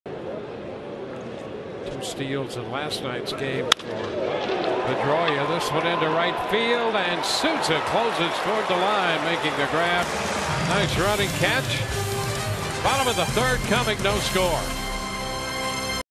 BOS@TB: Souza Jr. makes a running grab in right field
Steven Souza Jr. charges in and makes a nice running catch along the right-field line for the third out in the 3rd inning